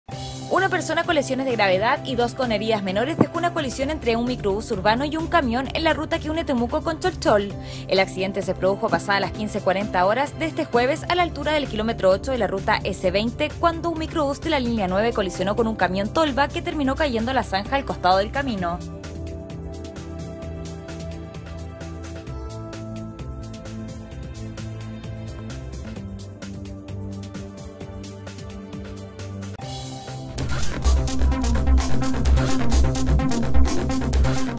Un lesionado grave dejó choque de microbus con camión en la ruta Chol Chol - Temuco
El accidente se produjo a la altura del kilómetro 8 de la Ruta S-20 y dejó tres personas lesionadas. Al lugar asistieron tres compañías de Bomberos, Carabineros y el Samu.